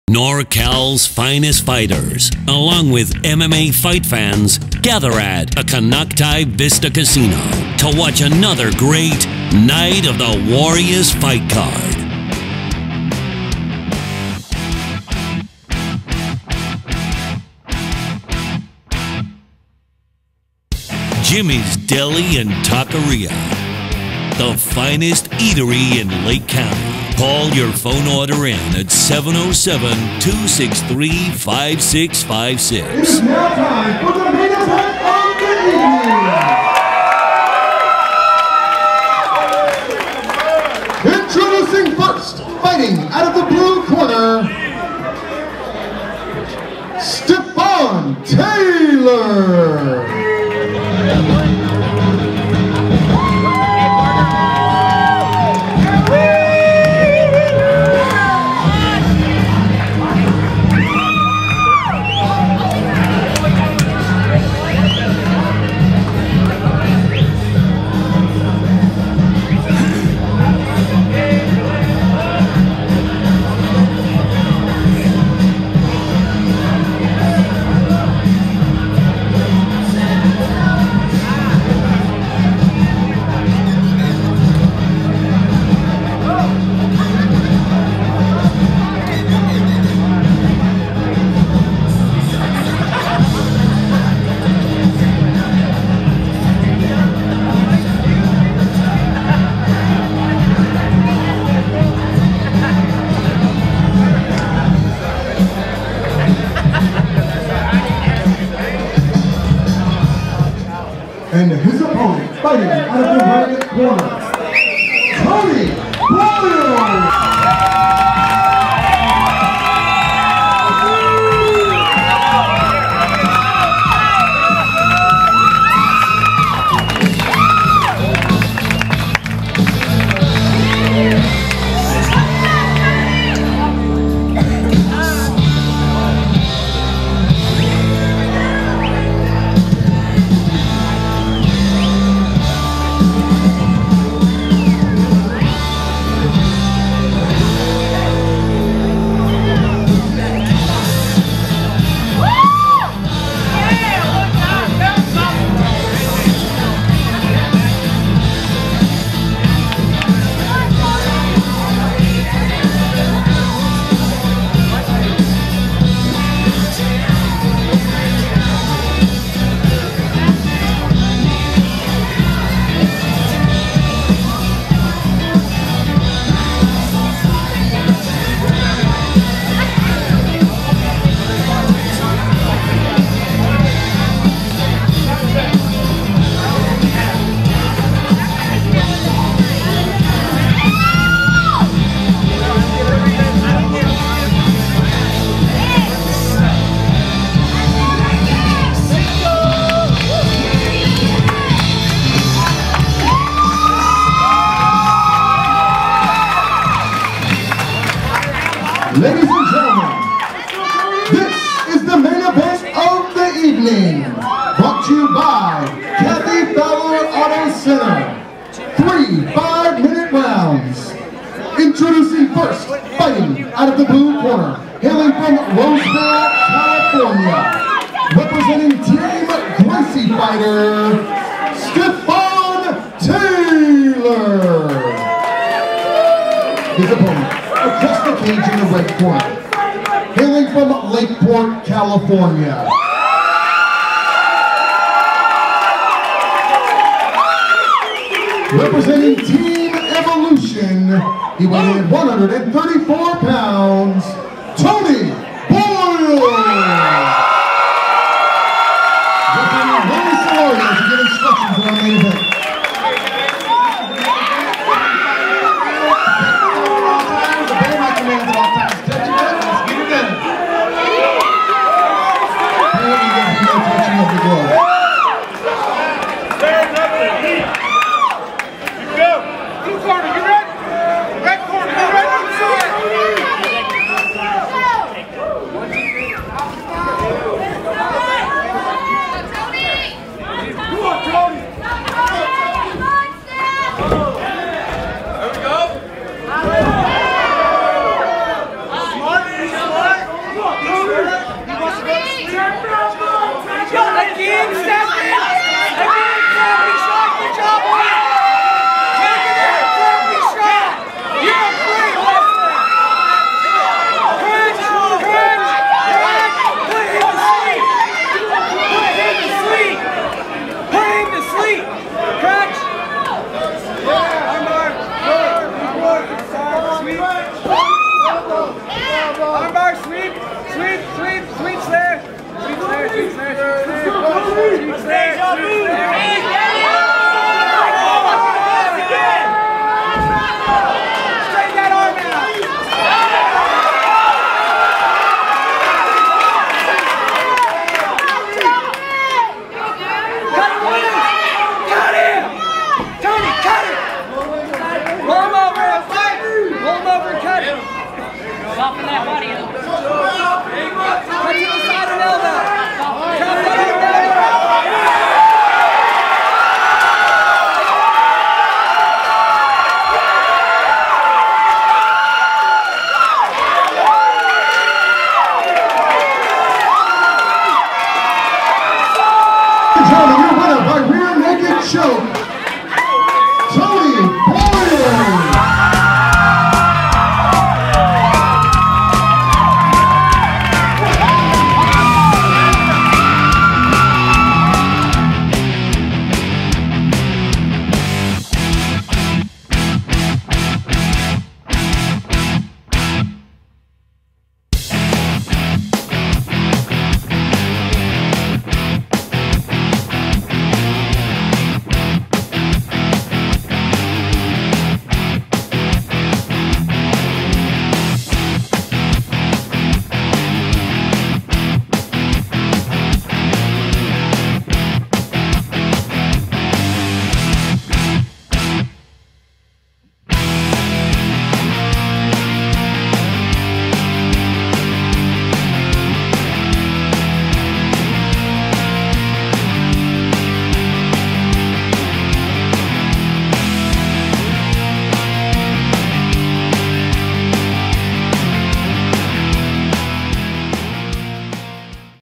Night of the Warriors 11 Stephone Taylor vs Tony Boyles. Sponsored by Kathy Fowler Auto Center.
WINNER: Boyles, Rnd – 1, 1:22, RNC,  Ref – Willy Solario

Rhythm Matters Studios specialize in action footage. Yet we love to play with events of all kinds. Give us a call for your future events.